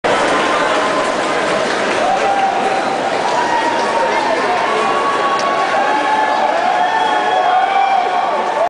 Mobile Clubbing - Liverpool Street 2
Further video from the Mobile Clubbing at Liverpool Street Station, people spread out and went for it. Lots of bemused commuters.